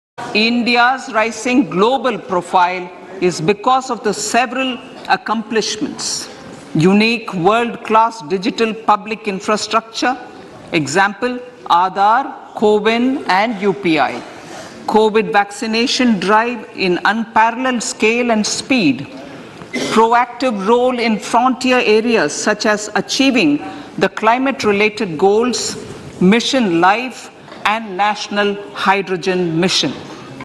भारत की बढ़ती वैश्विक प्रोफाइल कई उपलब्धियों, अद्वितीय विश्व स्तरीय सार्वजनिक बुनियादी ...
भारत की बढ़ती वैश्विक प्रोफाइल कई उपलब्धियों, अद्वितीय विश्व स्तरीय सार्वजनिक बुनियादी ढाँचे, जैसे- आधार, CoWIN और UPI के कारण है: केंद्रीय वित्त मंत्री @nsitharaman